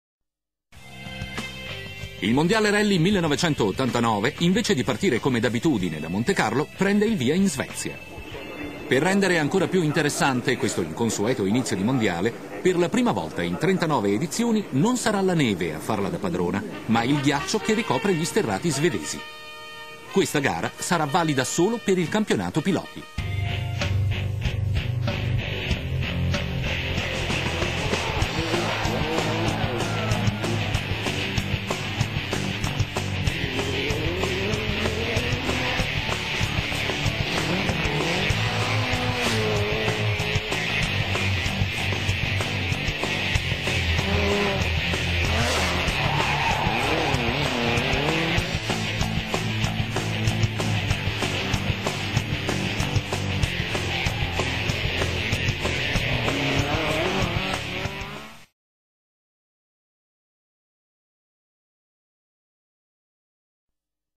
MONDIALE RALLY 1989 Miki Biasion e la Delta Integrale
Nellultimo mondiale degli anni 80 Miki Biasion e la Delta Integrale concessero uno strepitoso bis, dominando la stagione fin dalle prime battute.